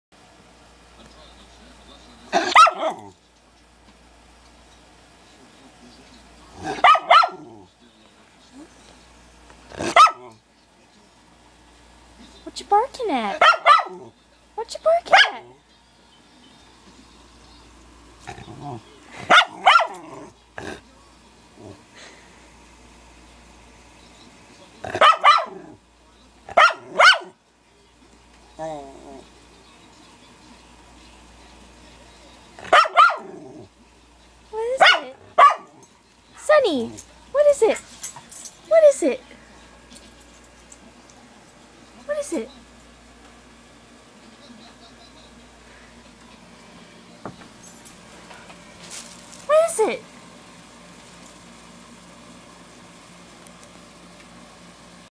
Sonny the Schnauzer finds himself
My dog Sonny barks at himself in the mirror then runs away.